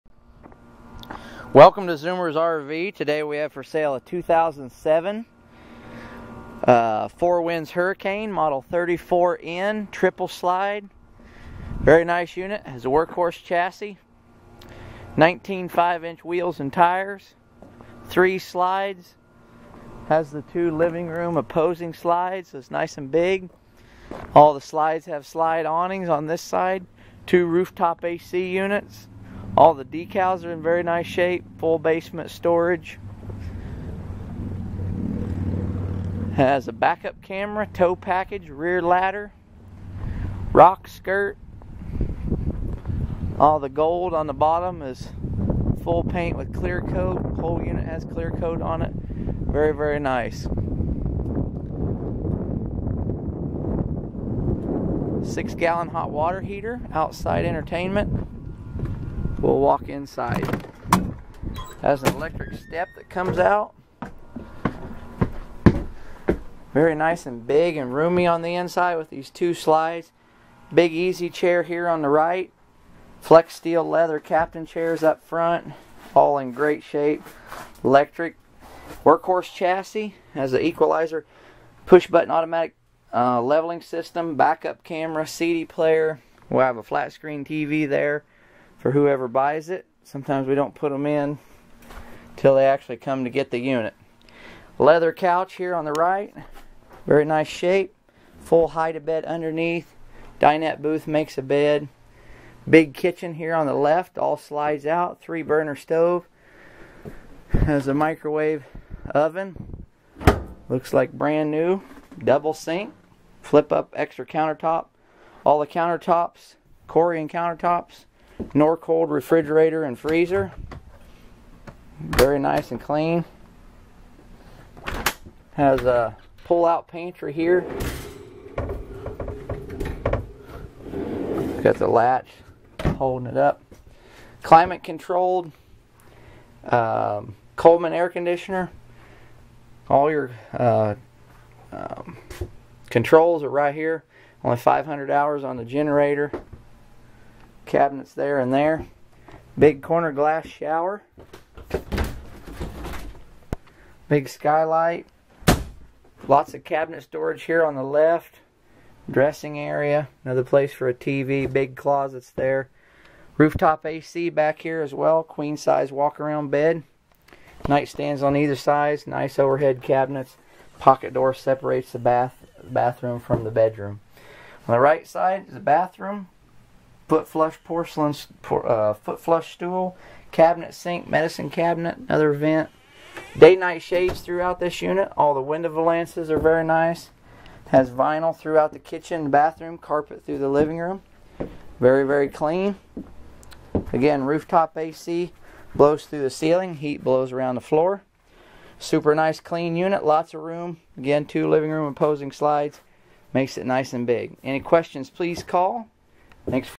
Nice Clean 34' 2007 Four Winds Hurricane 34N Workhorse Chassis 3-Slides!!
Nice Clean 34' 2007 Four Winds Hurricane 34N Workhorse Chassis 3-Slides!!  This Unit has a Great Floor Plan with 2 Living Room Opposing Slides that Makes it has Lots of Room in the Living Room.. It is on a Workhorse Chevrolet Chassis with Plenty of Power..  Loaded with Options.. New this Unit was $99,333 NADA Average Book $65,435 ZOOMERS SUPER LOW PRICE $36,900. Easy Financing SUPER LOW MONTHLY PAYMENT $327 NO MONEY DOWN with approved credit.